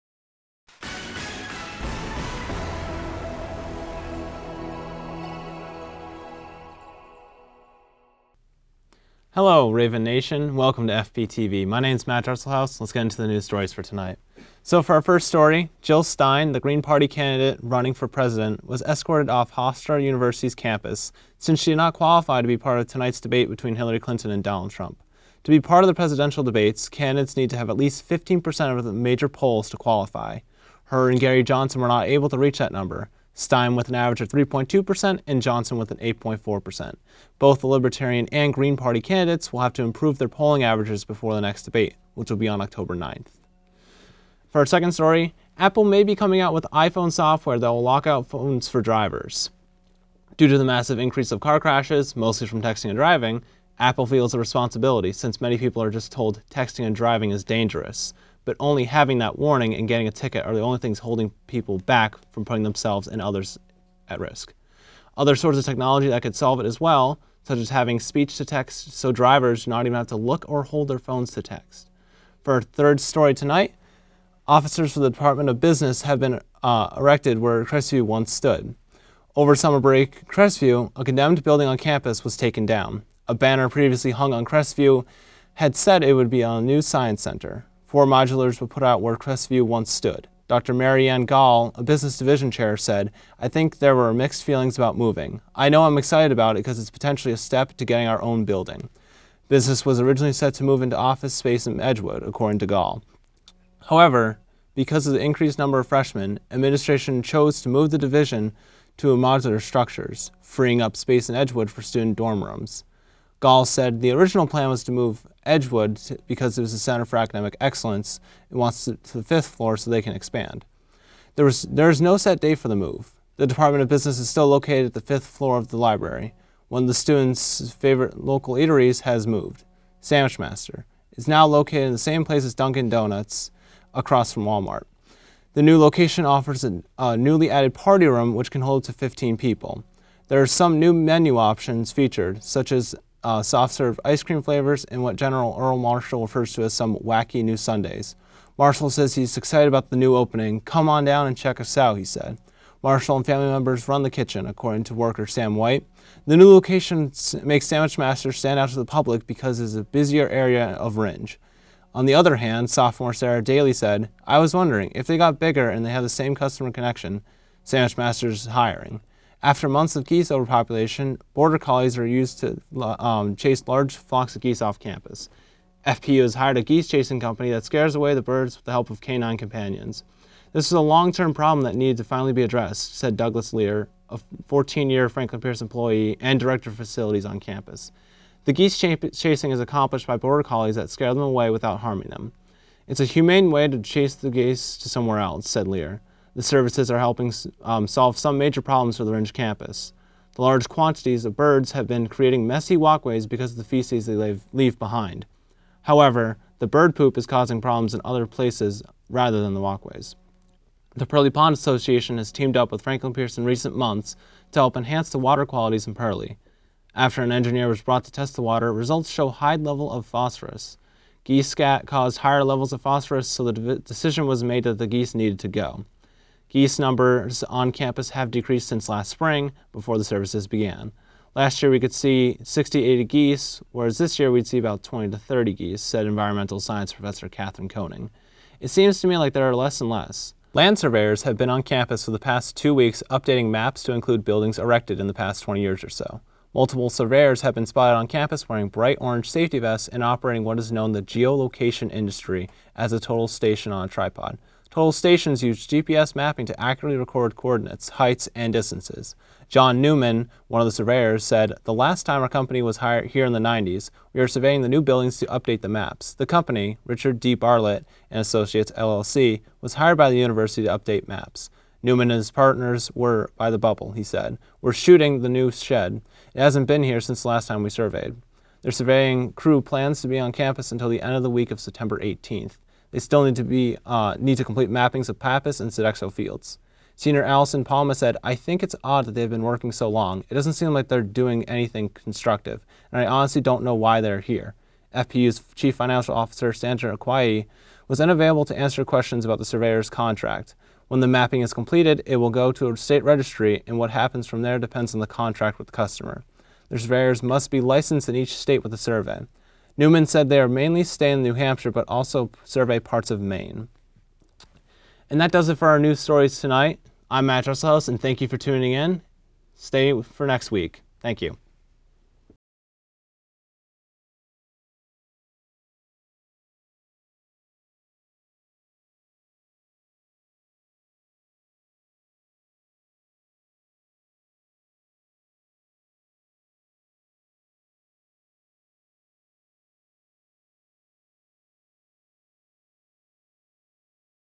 FPTV News 10/11/16 Episode 17
Franklin PIerce university students put on FPTV's 3rd newscast of the year!

Matt Dresselhouse talks whats happening here on campus and what's happening out in the world.